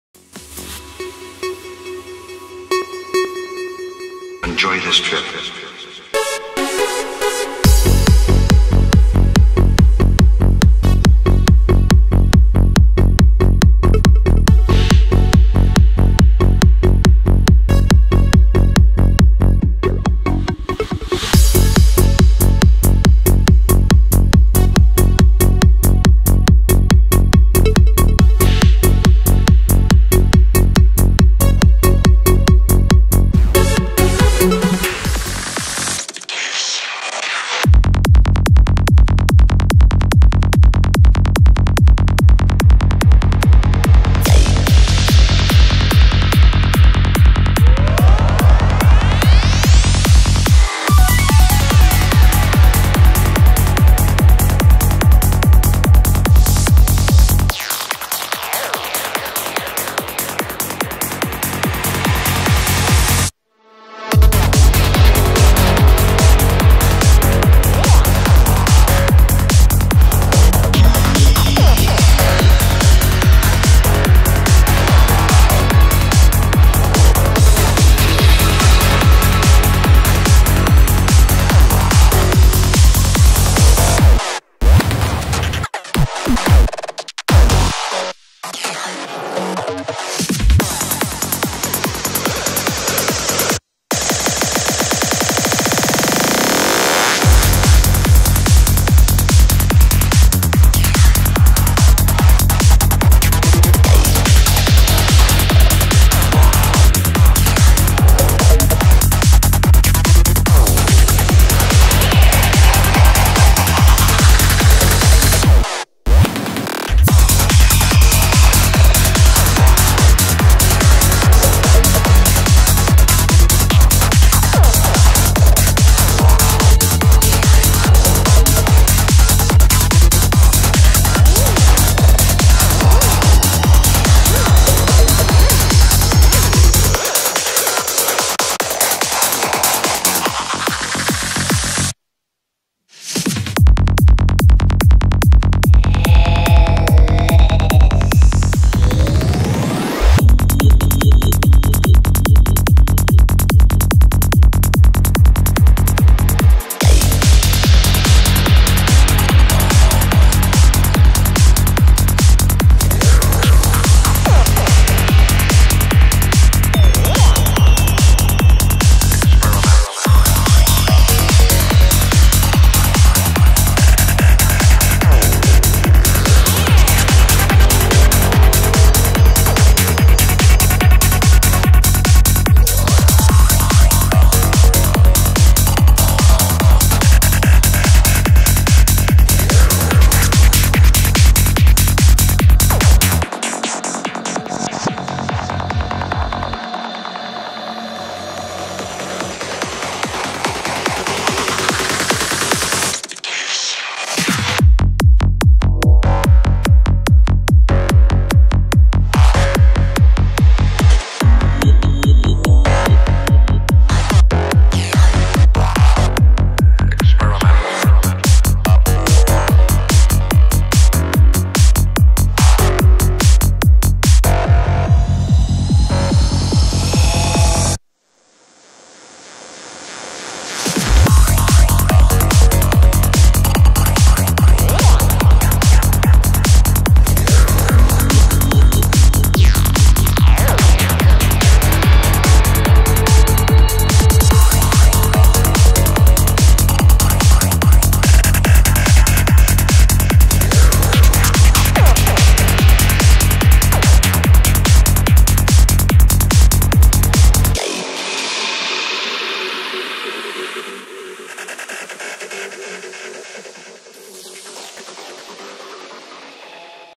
Bunker themed event
psytrance rave in a lost bunker in the Netherlands
Bunker Themed event by DJ SoundloopZ